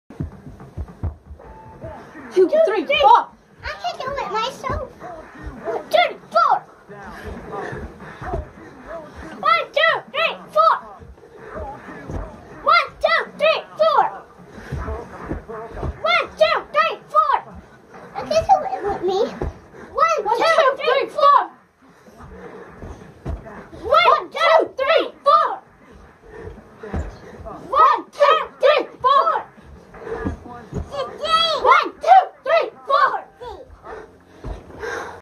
Beachbody Coaching, NOT just about MONEY
Being a Beachbody Coach is not always the easiest thing to do. But when your children decide to workout with you and want to do healthier things not because it is the thing to do but because they are learning young that it is normal, that's when being a coach means so much more than helping family, friends, and strangers. You are grooming the next generation.